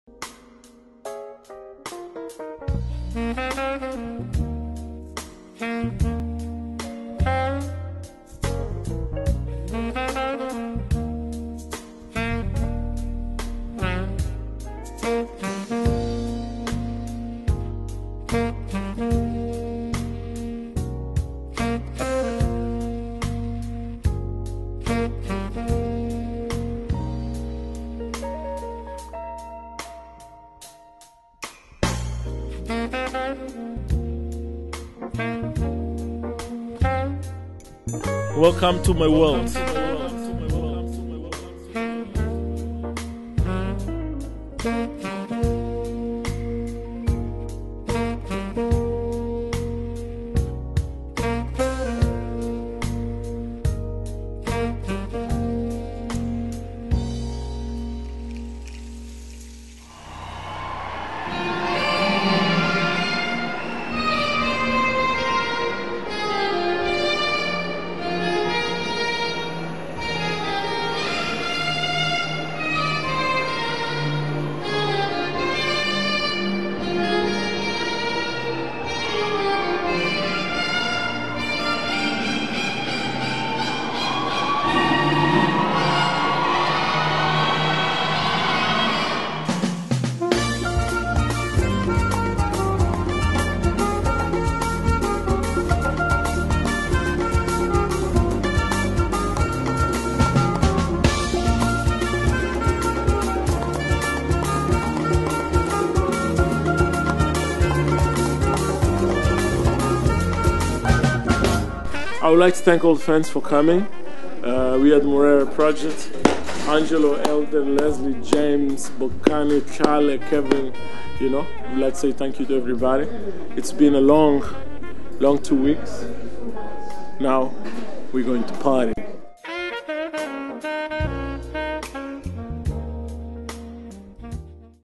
Moreira Chonguica - 13th Cape Town International Jazz Festival March 2012
This short video covers some of the action surrounding the preparation for the CTIJF by Moreira and the members of The Moreira Project such as rehearsals as well as an early morning appearance on the TV show Expresso and obviously at the concert itself on March 30, 2012 in Kippies.